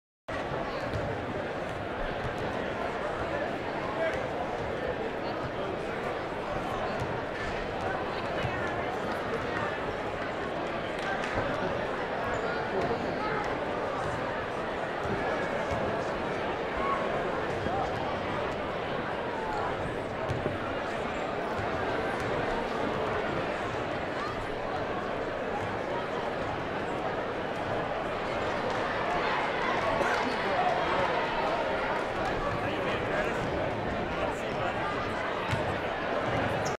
4 Year Old Shoots Hoops UNBELIEVABLE!!
Recruiters, I hope your're watching!!
Think you're good at basketball? WATCH THIS!!
half time 'show' at a high school basketball game